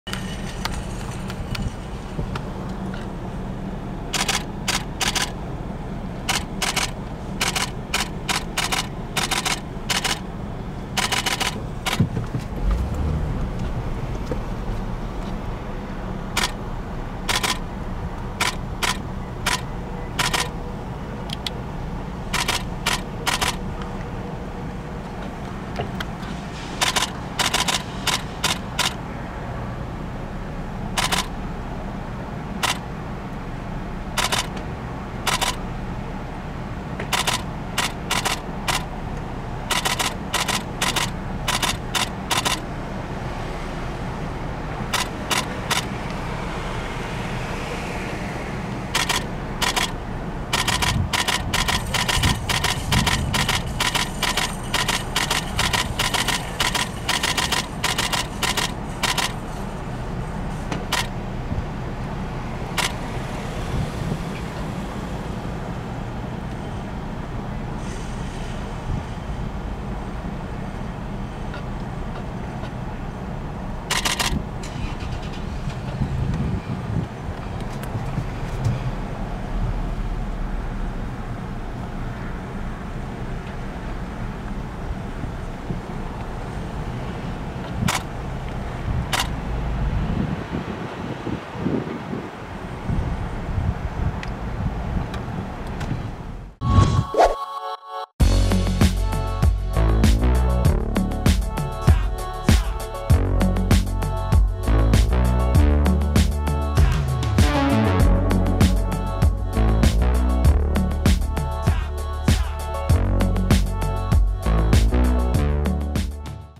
Brody Jenner Rocks Heavy Metal Gucci Shirt During Grocery Run In Calabasas
Brody Jenner is seen rocking a mask and Gucci heavy metal style t-shirt as he steps out at Erewhon Market in Calabasas for a few snacks. Jenner is accompanied by a pal during his grocery run and the duo leave in the same pick up truck.